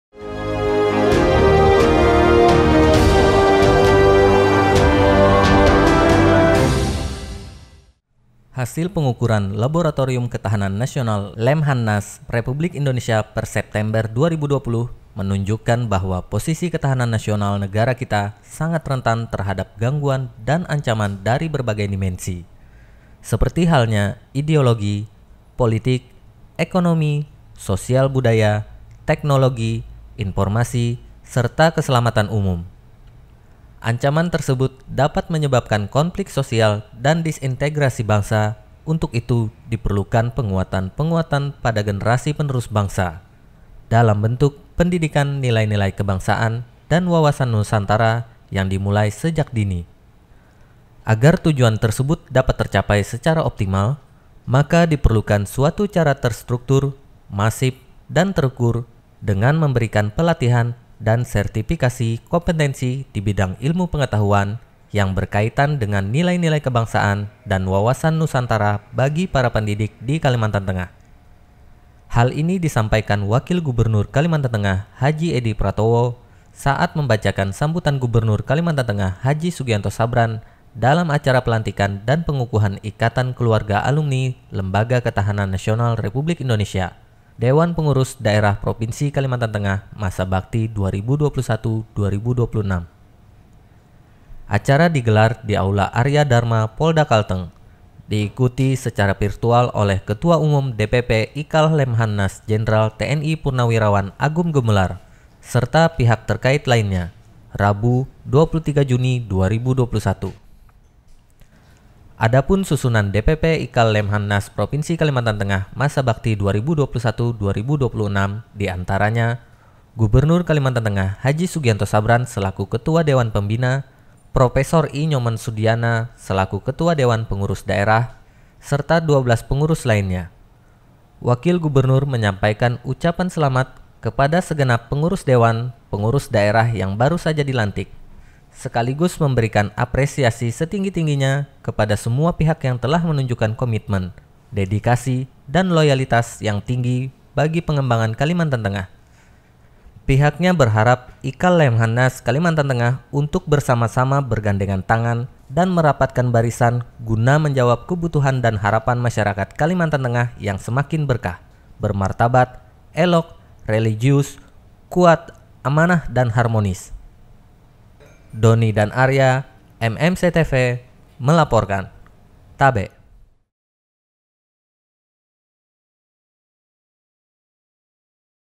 PELANTIKAN DAN PENGUKUHAN IKATAN KELUARGA ALUMNI LEMBAGA KETAHANAN NASIONAL REPUBLIK INDONESIA
HASIL PENGUKURAN LABORATORIUM KETAHANAN NASIONAL LEMHANNAS REPUBLIK INDONESIA PER SEPTEMBER 2020 MENUNJUKKAN BAHWA POSISI KETAHANAN NASIONAL NEGARA KITA SANGAT RENTAN TERHADAP GANGGUAN DAN ANCAMAN DARI BERBAGAI DIMENSI SEPERTI HALNYA IDEOLOGI, POLITIK, EKONOMI, SOSIAL BUDAYA, TEKNOLOGI, INFORMASI SERTA KESELAMATAN UMUM.

ANCAMAN TERSEBUT DAPAT MENYEBABKAN KONFLIK SOSIAL DAN DISINTEGRASI BANGSA UNTUK ITU DIPERLUKAN PENGUATAN PENGUATAN PADA GENERASI PENERUS BANGSA DALAM BENTUK PENDIDIKAN NILAI-NILAI KEBANGSAAN DAN WAWASAN NUSANTARA YANG DIMULAI SEJAK DINI.

AGAR TUJUAN TERSEBUT DAPAT TERCAPAI SECARA OPTIMAL, MAKA DIPERLUKAN SUATU CARA TERSTRUKTUR MASIF DAN TERUKUR DENGAN MEMBERIKAN PELATIHAN DAN SERTIFIKASI KOMPETENSI DI BIDANG ILMU PENGETAHUAN YANG BERKAITAN DENGAN NILAI-NILAI KEBANGSAAN DAN WAWASAN NUSANTARA BAGI PARA PENDIDIK DI KALIMANTAN TENGAH.

HAL INI DISAMPAIKAN WAKIL GUBERNUR KALIMANTAN TENGAH HAJI EDI PRATOWO SAAT MEMBACAKAN SAMBUTAN GUBERNUR KALIMANTAN TENGAH HAJI SUGIANTO SABRAN DALAM ACARA PELANTIKAN DAN PENGUKUHAN IKATAN KELUARGA ALUMNI LEMBAGA KETAHANAN NASIONAL REPUBLIK INDONESIA DEWAN PENGURUS DAERAH PROVINSI KALIMANTAN TENGAH MASA BAKTI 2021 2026.

ACARA DIGELAR DI AULA ARYA DHARMA POLDA KALTENG DAN DIIKUTI SECARA VIRTUAL OLEH KETUA UMUM DPP IKAL LEMHANAS JENDRAL TNI PURNAWIRAWAN AGUM GUMELAR SERTA PIHAK TERKAIT LAINNYA, RABU 23 JUNI 2021.

ADAPUN SUSUNAN DPP IKAL LEMHANAS PROVINSI KALIMANTAN TENGAH MASA BAKTI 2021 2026 DIANTARANYA GUBERNUR KALIMANTAN TENGAH HAJI SUGIANTO SABRAN SELAKU KETUA DEWAN PEMBINA PROFESOR I NYOMAN SUDYANA SELAKU KETUA DEWAN PENGURUS DAERAH SERTA 12 PENGURUS LAINNYA.

WAKIL GUBERNUR MENYAMPAIKAN UCAPAN SELAMAT KEPADA SEGENAP PENGURUS DEWAN PENGURUS DAERAH YANG BARU SAJA DILANTIK SEKALIGUS MEMBERIKAN APRESIASI SETINGGI-TINGGINYA KEPADA SEMUA PIHAK YANG TELAH MENUNJUKKAN KOMITMEN DEDIKASI DAN LOYALITAS YANG TINGGI BAGI PENGEMBANGAN KALIMANTAN TENGAH.

PIHAKNYA JUGA BERHARAP UNTUK ITU IKAL LEMHANNAS KALIMANTAN TENGAH UNTUK BERSAMA SAMA BERGANDENGAN TANGAN DAN MERAPATKAN BARISAN GUNA MENJAWAB KEBUTUHAN DAN HARAPAN MASYARAKAT KALIMANTAN TENGAH YANG SEMAKIN BERKAH BERMARTABAT, ELOK, RELIGIUS, KUAT, AMANAH DAN HARMONIS.